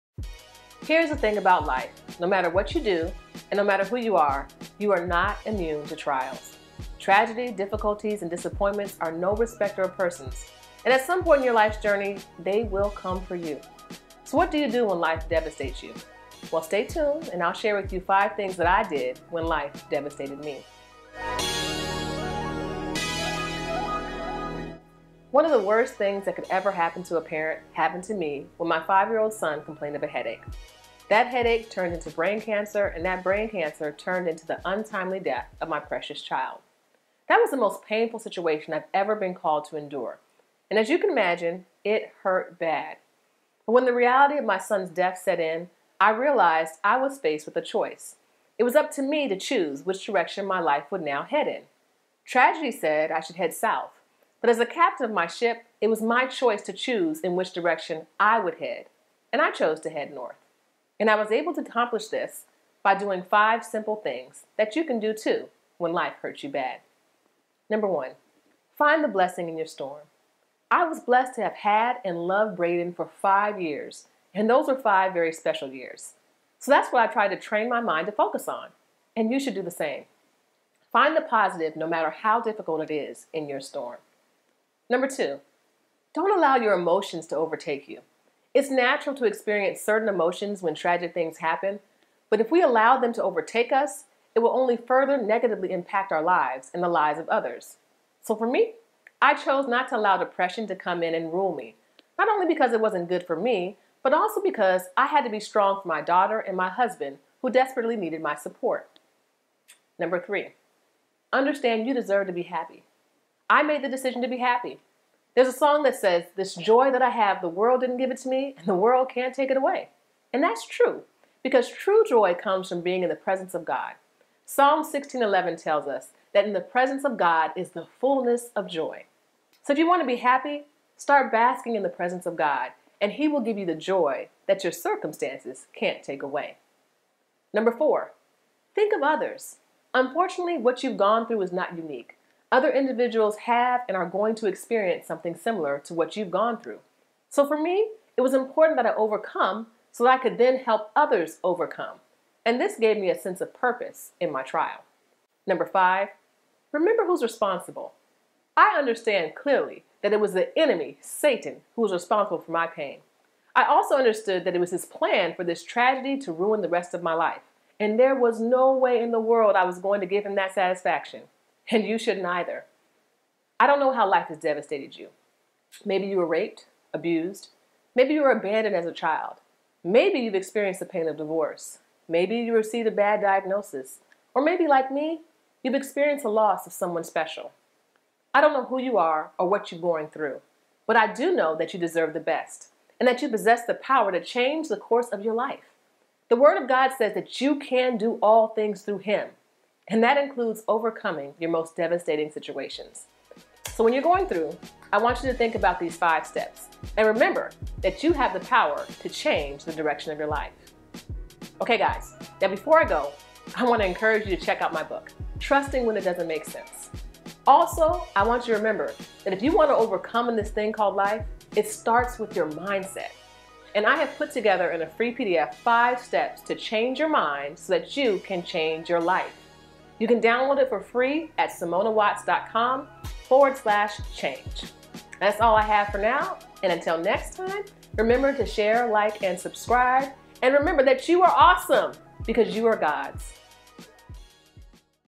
What Do You Do When Life Devastates You?
Life is tough and bad things happen to good folks and bad folks.  In this video I share some things I did to get through my most challenging season.

God bless and welcome to the Extraordinary Living Community! 🎉🎉